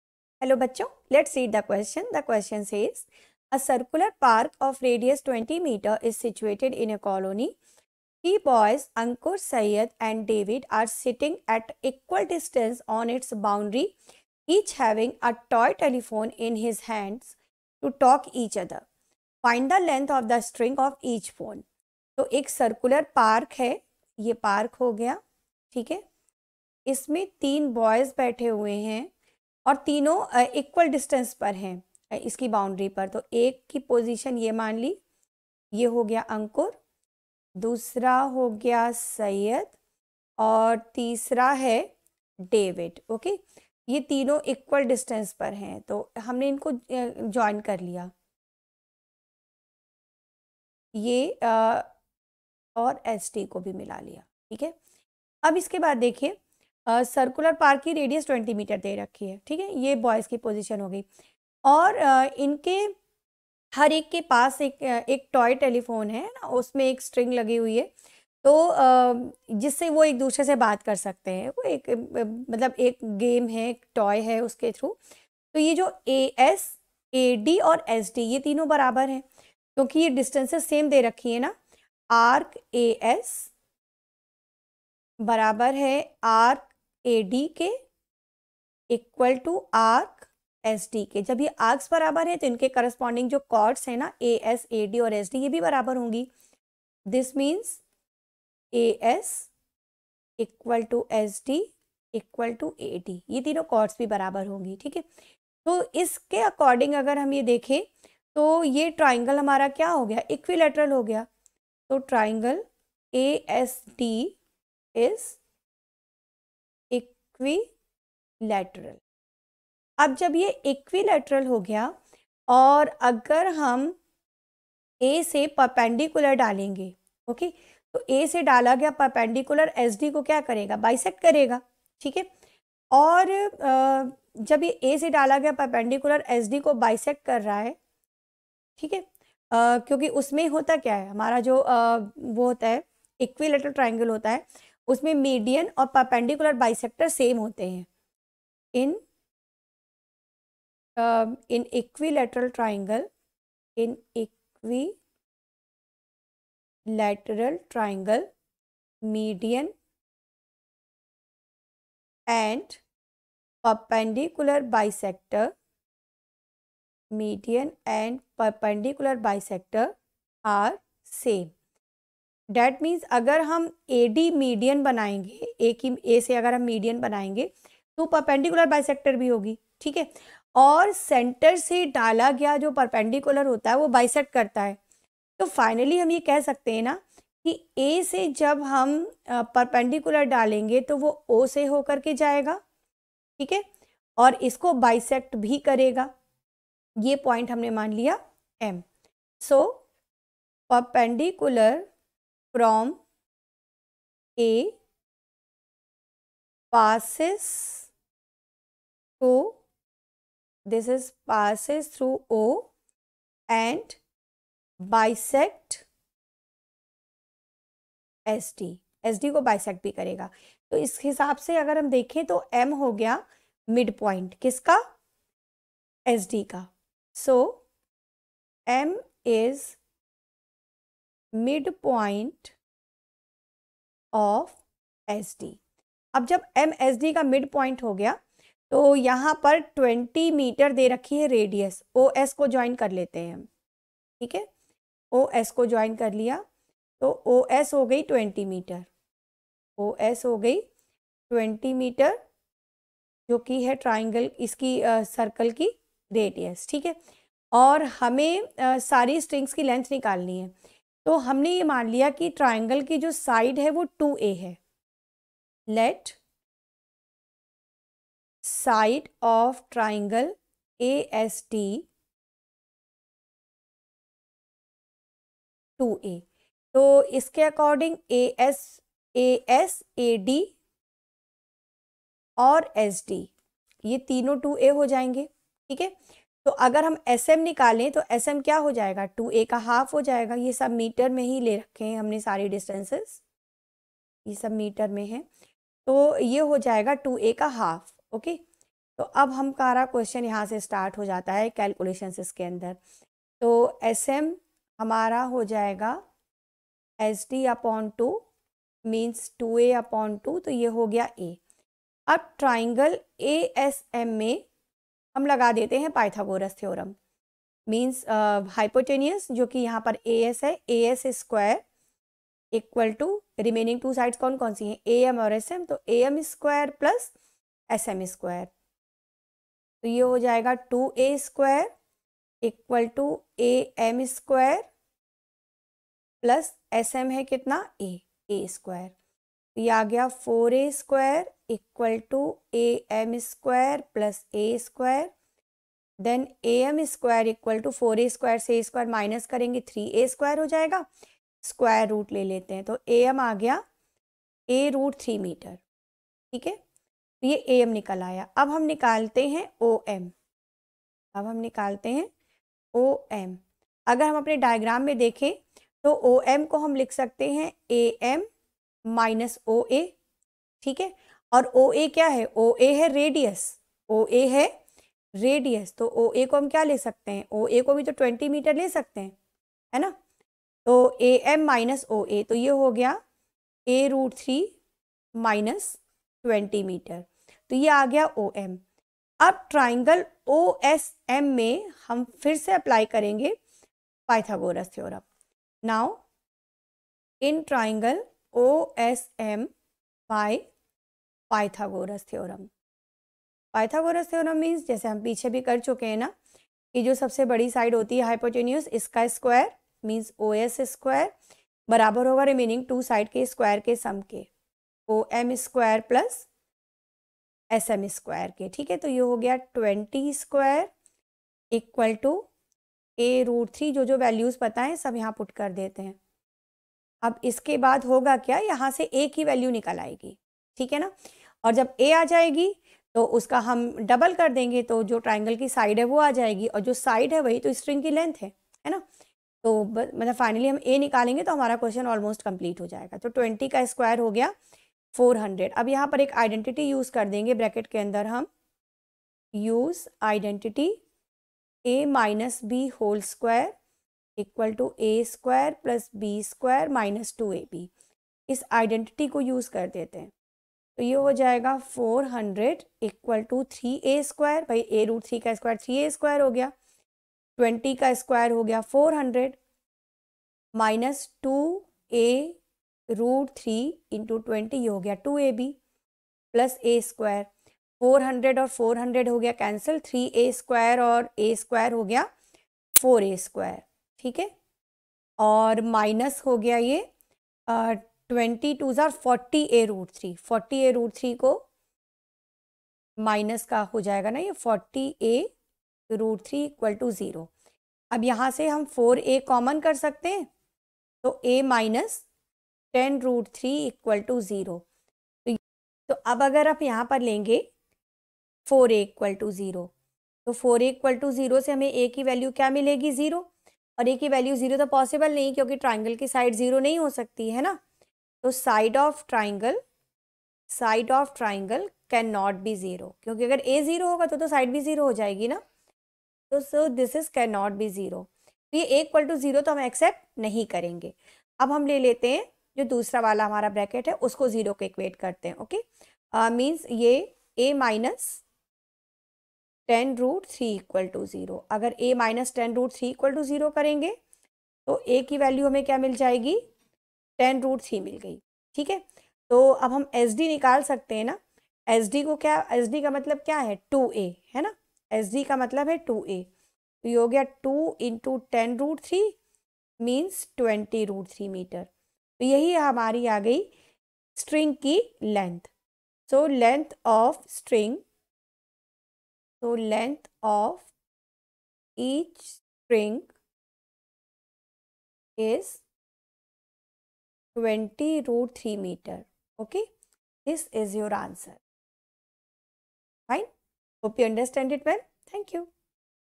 A circular park of radius 20 m is situated in a colony. Three boys Ankur, Syed and David are sit...
A circular park of radius 20  m is situated in a colony. Three boys Ankur, Syed and David are sitting at equal distance on its boundary each having a toy telephone in his hands to talk each other. Find the length of the string of each phone.
P